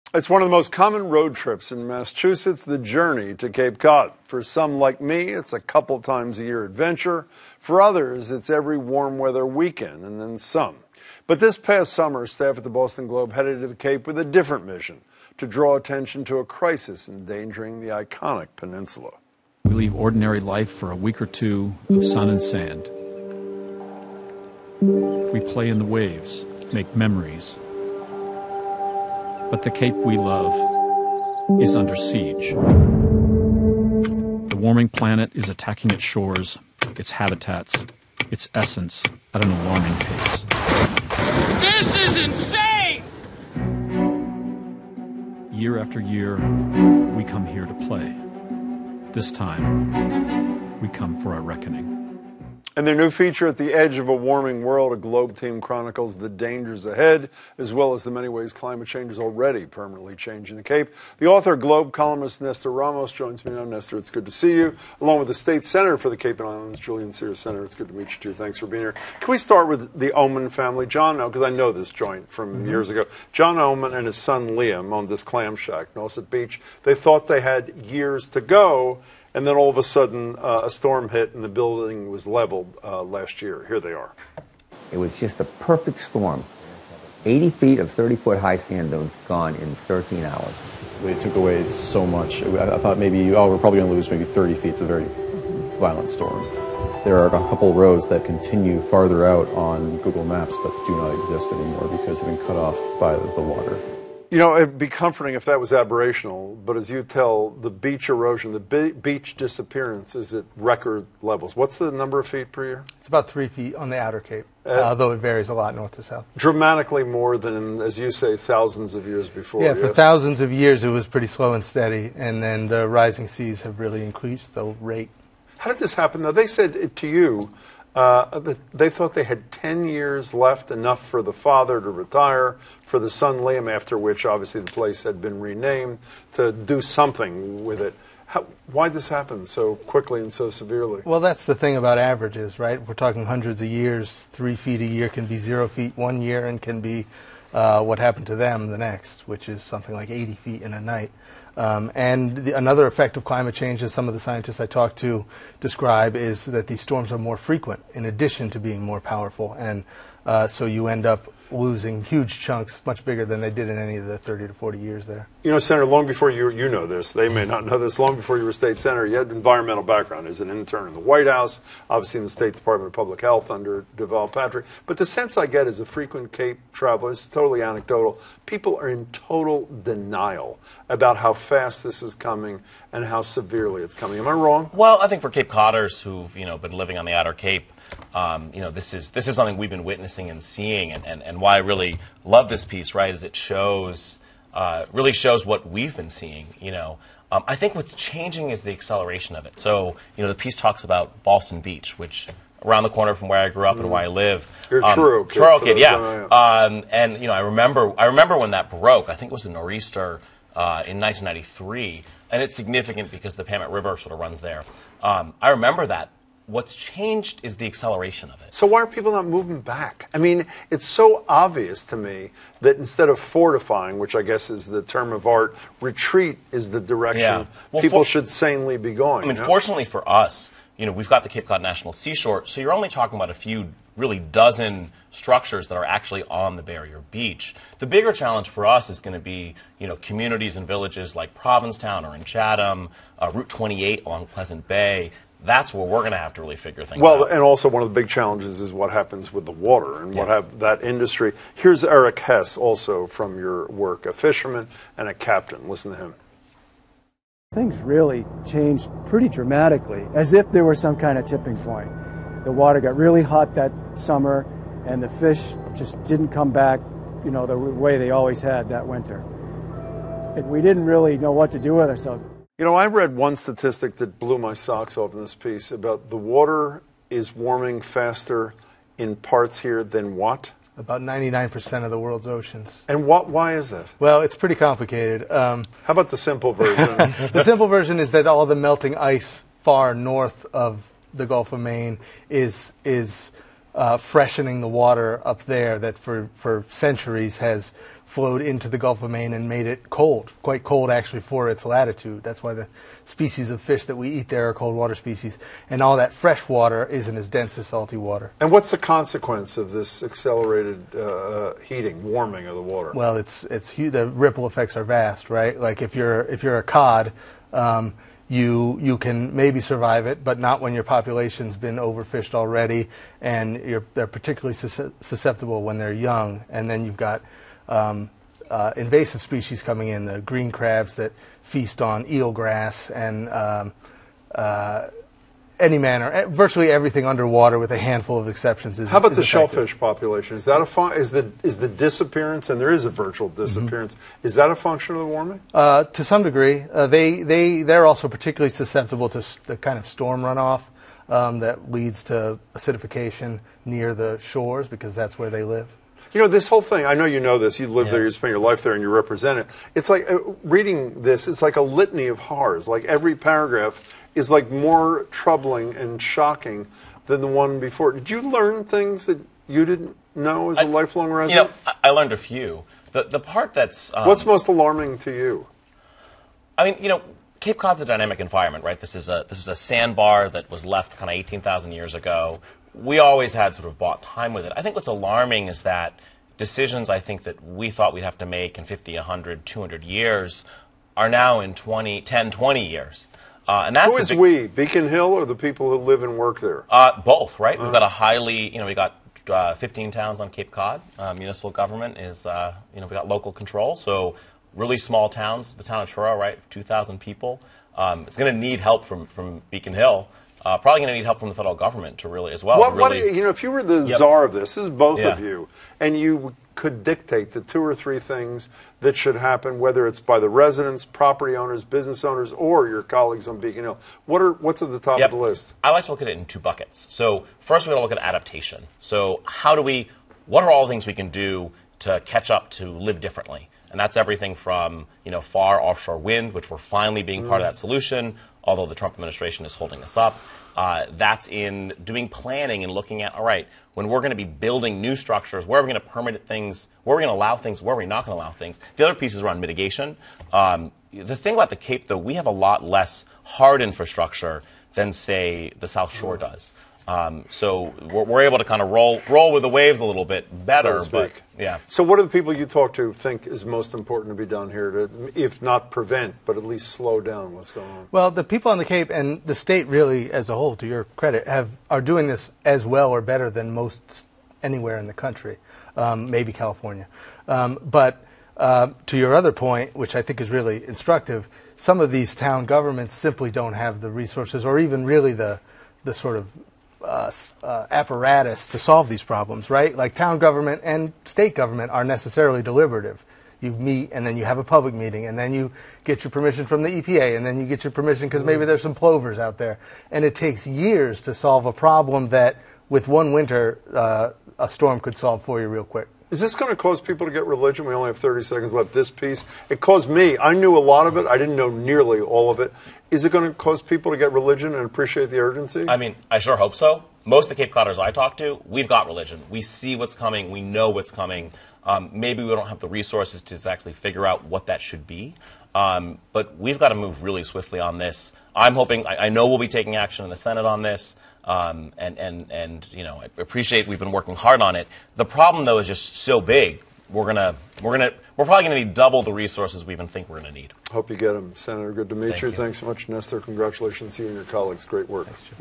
Tracking The Impacts Of Climate Change On Cape Cod
A new multimedia piece from the Boston Globe, ‘At the Edge of a Warming World,’ chronicles the dangers facing the region as climate change leads to warmer waters, changing weather patterns, and increased stresses on native wildlife. Jim Braude was joined by columnist Nestor Ramos, who authored the piece, and Massachusetts state senator for the Cape and Islands, Sen. Julian Cyr.